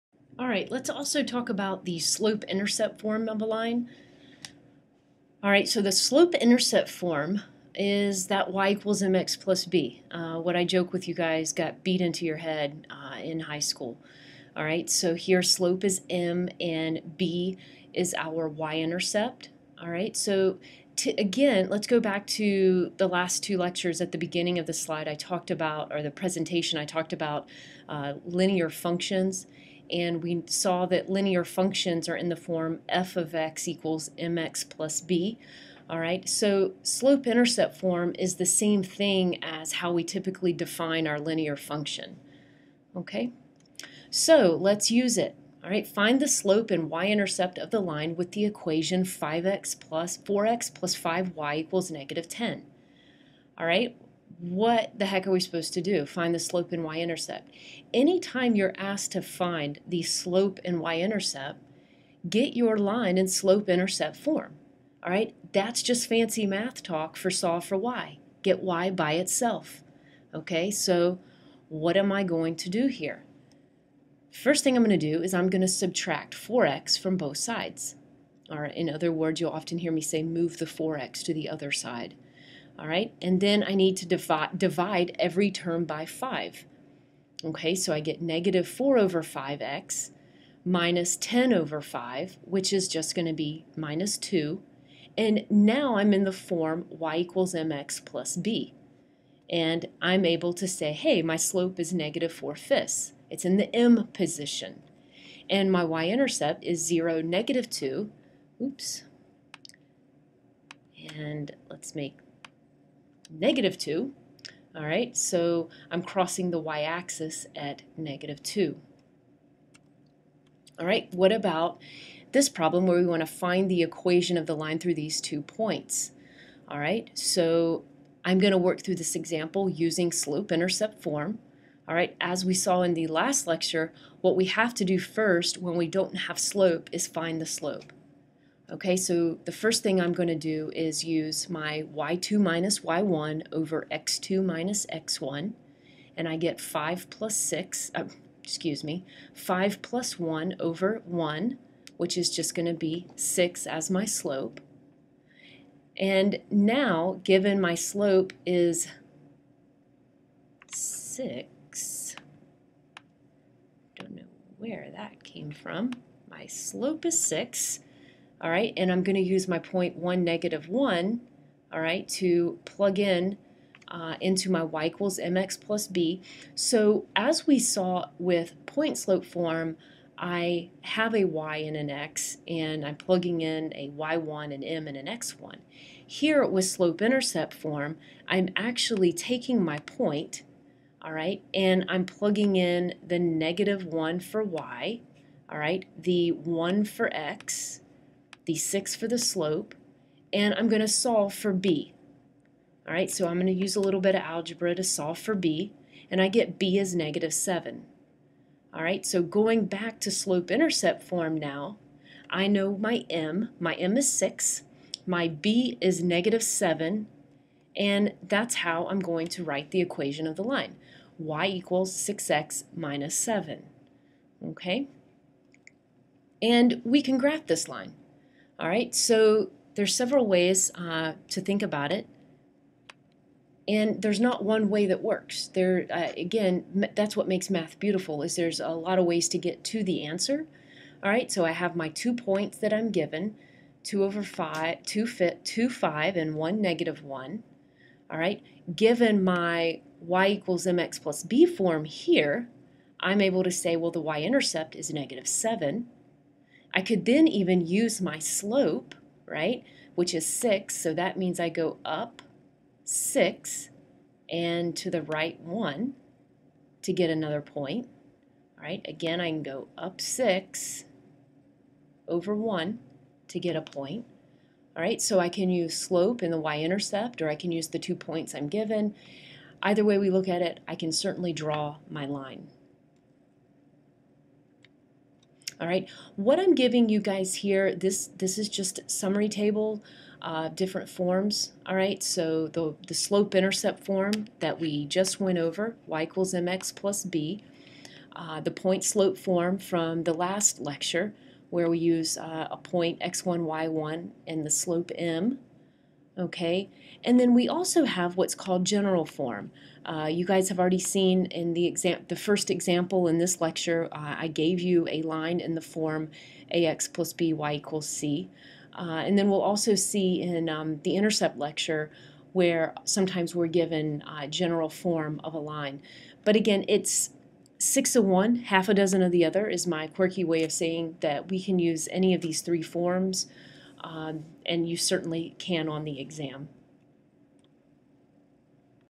Lecture - Slope-Intercept Form
Table of Contents:

00:00 - Slope-Intercept Form
00:04 -
00:43 - Using Slope-Intercept Form
01:59 - Using Slope-Intercept Form
02:35 - Using Slope-Intercept Form
03:36 - Using Slope-Intercept Form
04:44 - Summary of Forms for the Equation of a Line
05:06 - Summary of Forms for the Equation of a Line
05:29 - Summary of Forms for the Equation of a Line
05:36 - Summary of Forms for the Equation of a Line